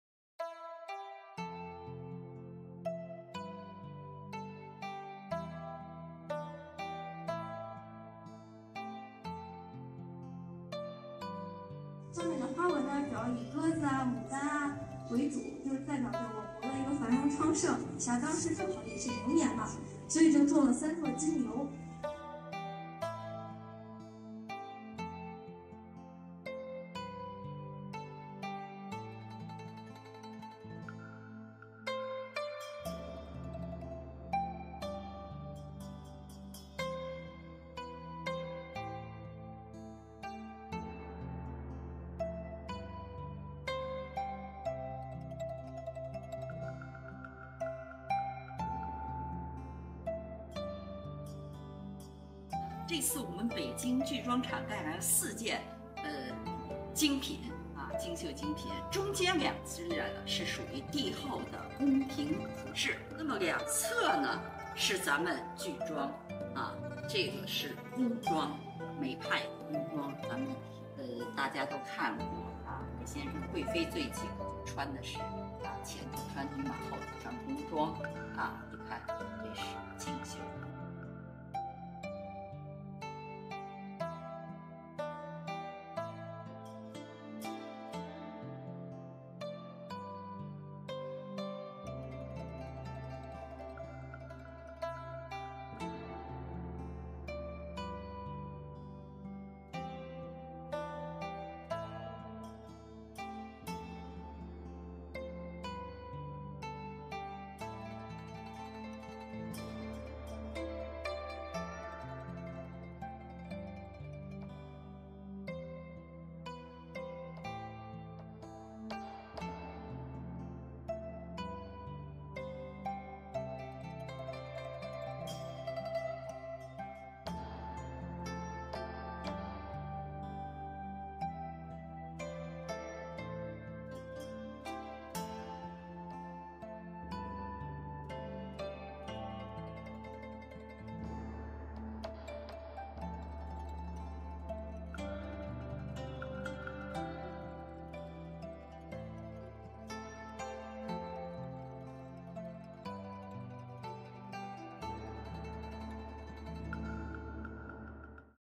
Exhibition of Chinese Cultural Relics
An exhibition that brings together intangible cultural heritages from China's southern and northern regions is ongoing in the National Center for the Performing Arts (NCPA) in Beijing and will run until April 16. 220 items of more than 30 outstanding intangible cultural heritages from Beijing and East China's Zhejiang province are on display.